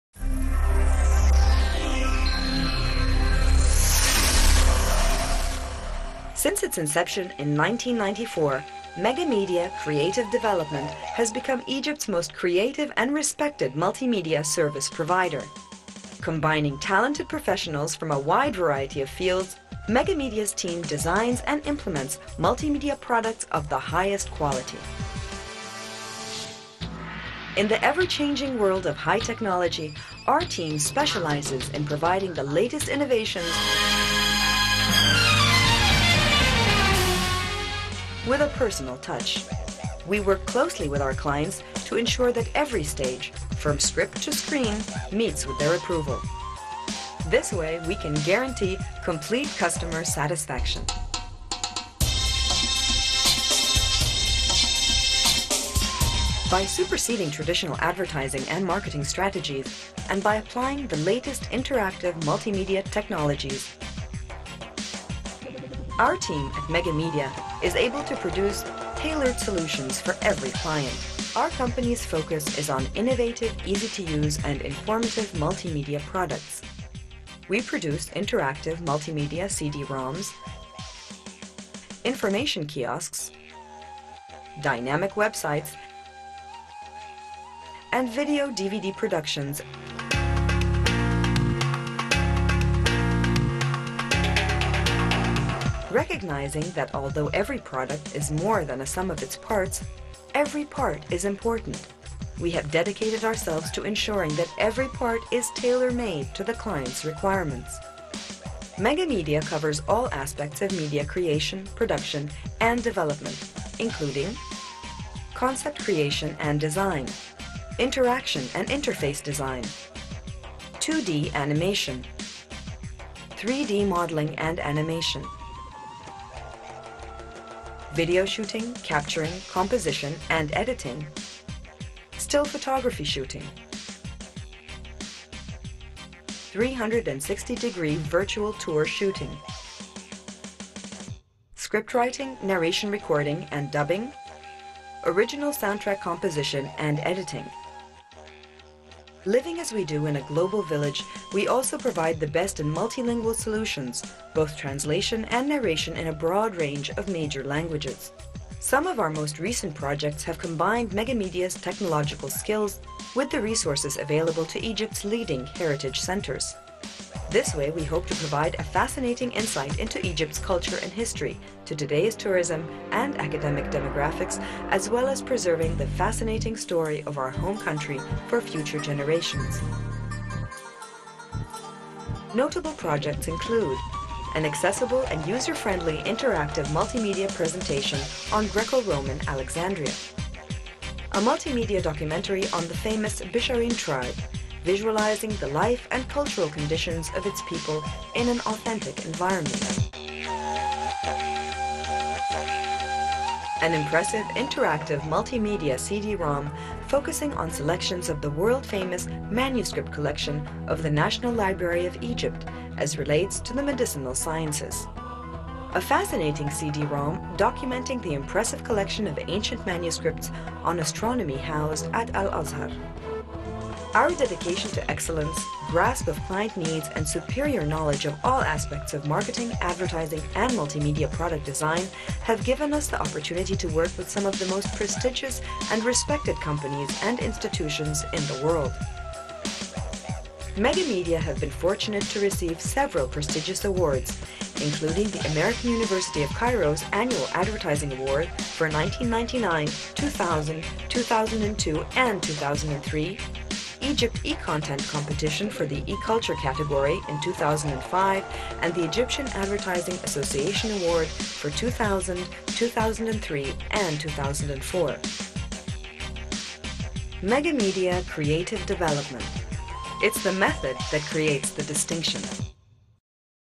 Mega Media Creative Development
Since its foundation in 1994, MEGA MEDIA has become Egypt's most innovative & prestigious multi-media service provider. A dedicated team of highly talented creative professionals is planning and carrying out all stages of media design and production to guarantee highest possible customer satisfaction.